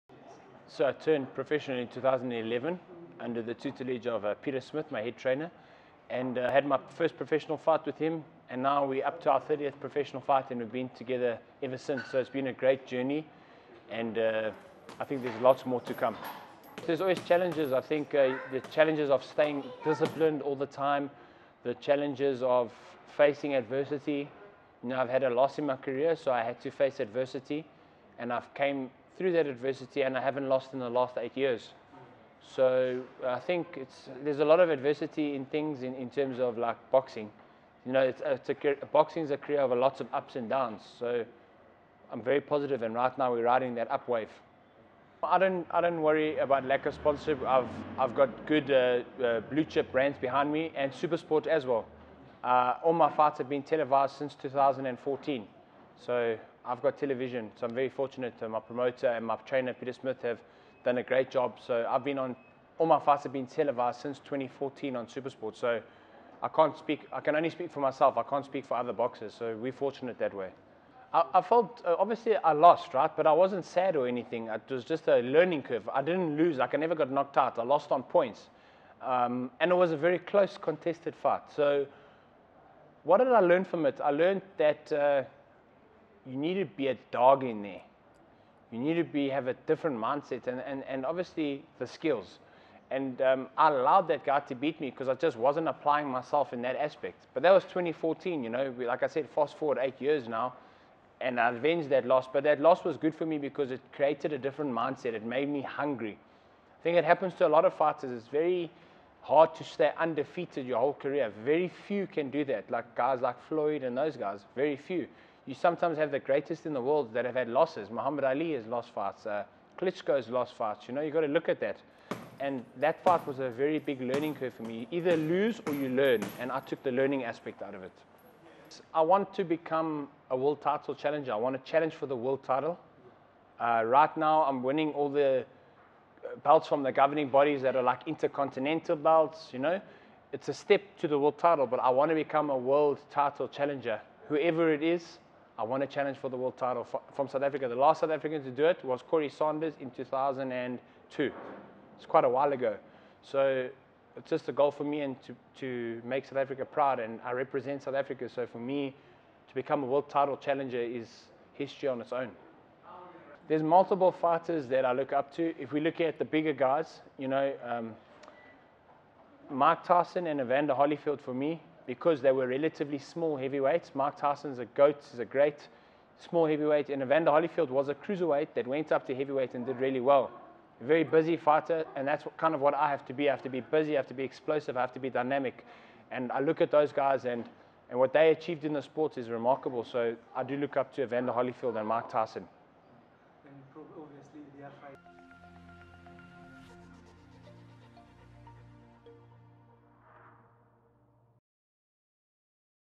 Kevin Lerena IBO heavyweight champion
South Africa’s Kevin Lerena is the new IBO Intercontinental Heavyweight champion after he defeated Polish giant Mariusz Wach at Emperors Palace in Johannesburg on Saturday